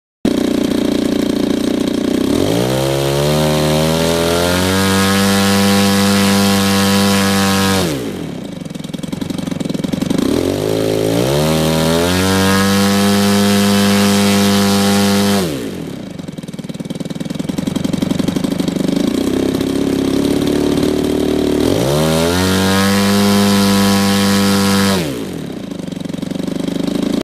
A6M Zero Fuji 86CC twin cylinder engine test with original carborator
Getting around 6,000 RPM's with a 24X10 Mejzlik CF prop.